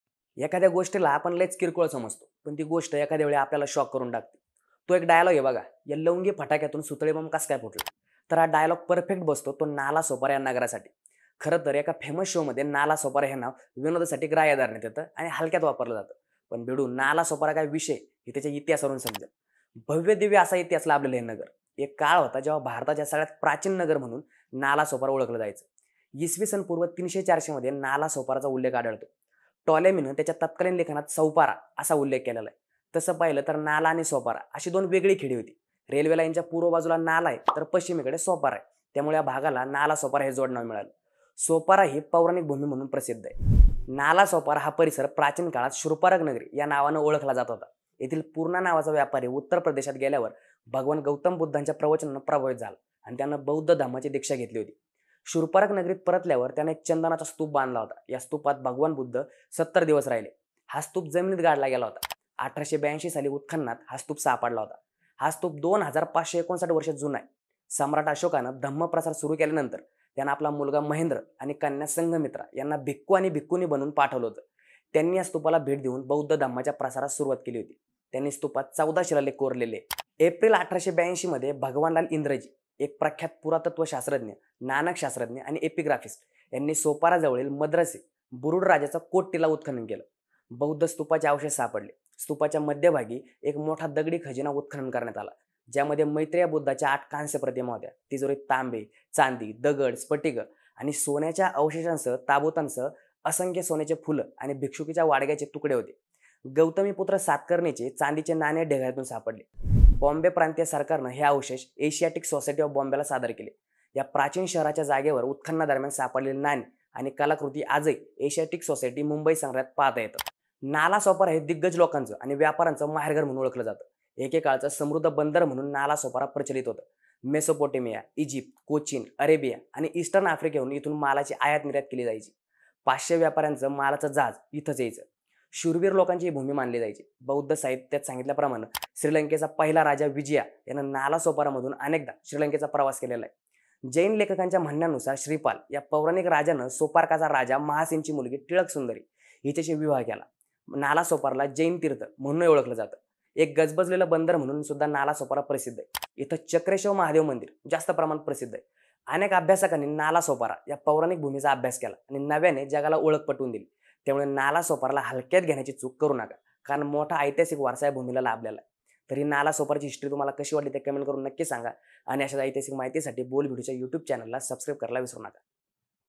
विषय फक्त आत्ताचा नाही, Nalasopara चा इतिहास इसवीसन पूर्व ३०० वर्ष जुना आहे I Bol Bhidu।
एखादया गोष्टीला आपण लईच किरकोळ समजतो पण ती गोष्ट एखादया वेळी आपल्यालाच शॉक करून टाकते. तो एक डायलॉग आहे बघा की या लवंगी फटाक्यातून सुतळी बॉम्ब कस काय फुटला ? तर हा डायलॉग परफेक्ट बसतो तो नालासोपारा या नगरासाठी.

खरंतर एका फेमस शोमध्ये नालासोपारा हे नाव विनोदासाठी ग्राह्य धरण्यात येत आणि हलक्यात वापरलं जातं पण भिडू नालासोपारा काय विषय आहे हे व्हिडिओ बघून समजेल. भव्य दिव्य असा इतिहास लाभलेलं हे नगर. एक काळ होता जेव्हा भारतात सगळयात प्राचीन नगर म्हणून नालासोपारा ओळखलं जायचं.

Actually in a famous show the name Nalasopara is taken for joke and used lightly but after watching the video you will understand what Nalasopara is all about. This city has a rich and glorious history. There was a time when Nalasopara was known as the oldest city in India.